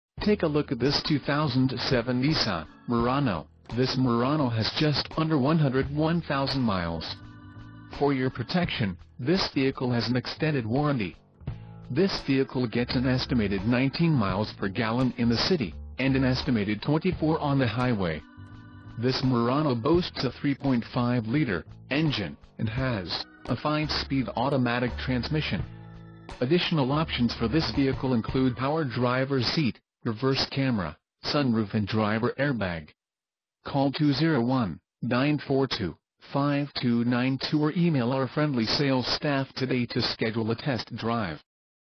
2007 Nissan Murano AWD 4dr SL SUV - Jersey City, NJ
New Jersey State Auto Auction, Jersey City, NJ - Nissan Murano SL AWD Leather Sunroof Reverse Camera